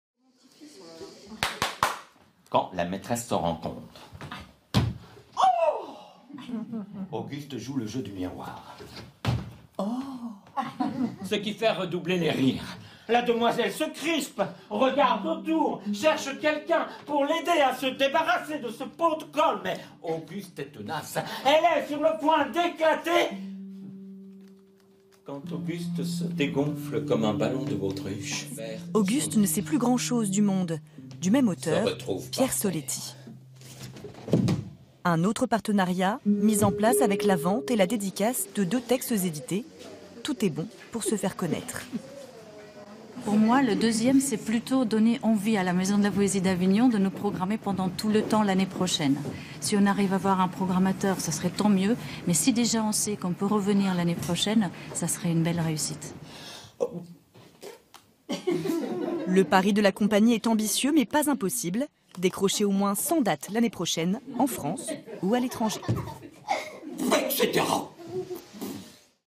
Pierre Soletti | Auguste ne sait plus grand-chose du monde (théâtre)
Reportage France 3 | Festival OFF d'Avignon 2017

Mise en scène : Mateja Bizjak Petit
Texte : Pierre Soletti
Interprétation : Pascal Thétard